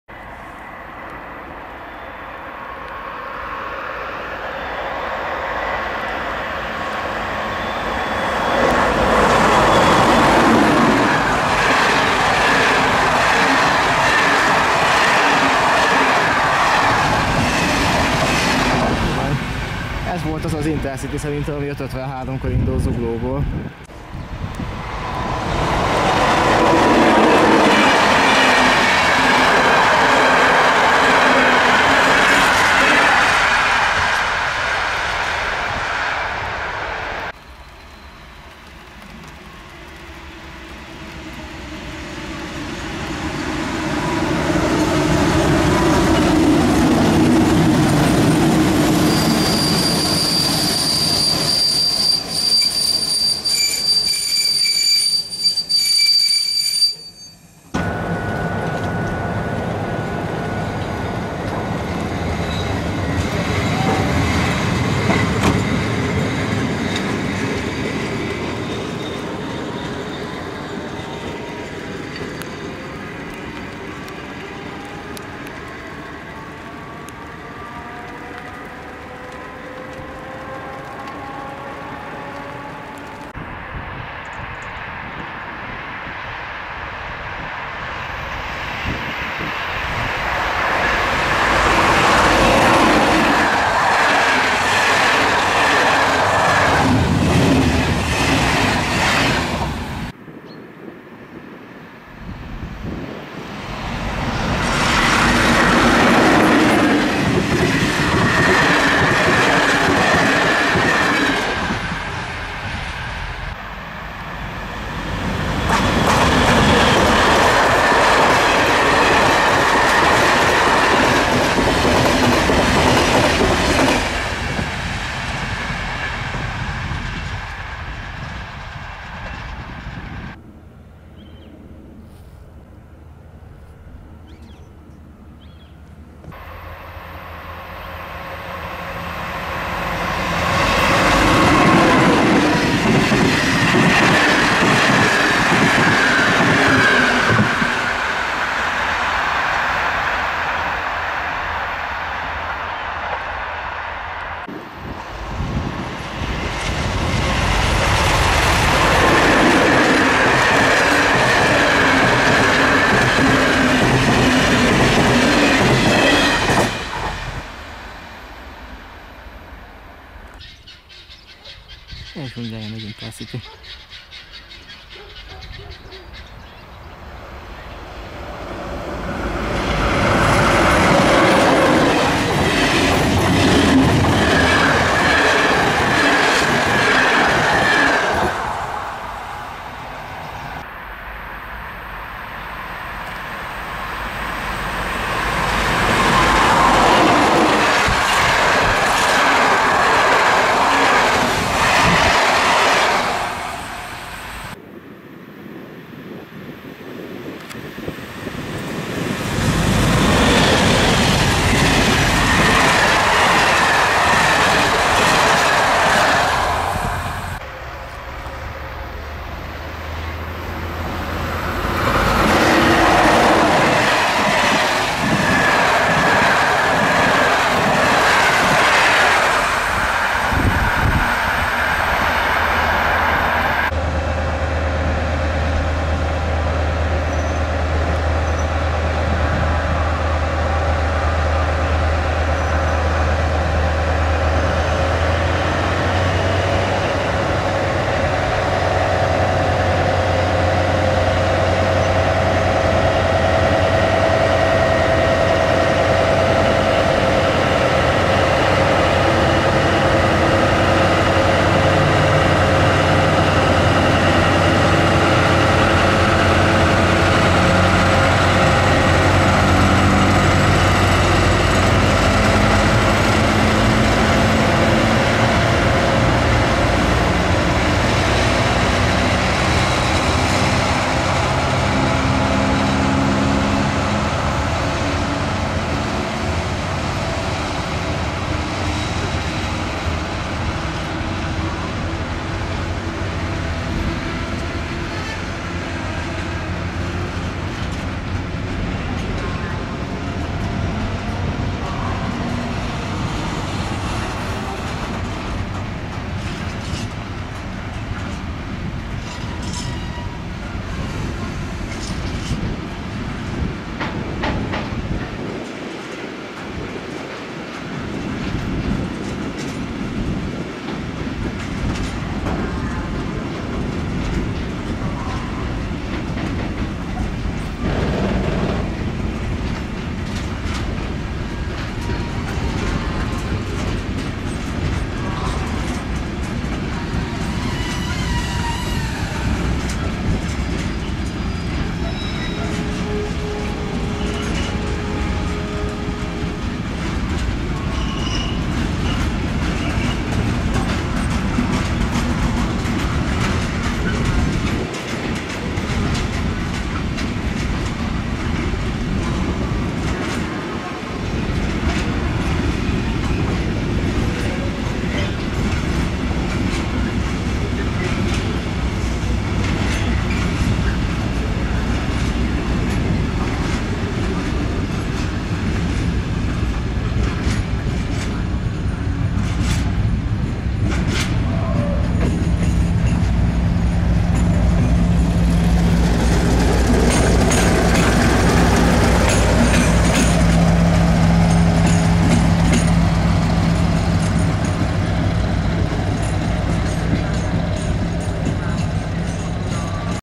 2022.04 Trainspotting 140-es vonal
Bankszámla:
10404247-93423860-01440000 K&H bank

Nyíltvonali trainspotting videók a 140-es vonalról Kiskunfélegyháza - Szatymaz közötti szakaszán nyíltvonalon több helyen is. Sajnos csak a menetrend volt aznap és egy CER vonat Félegyházán.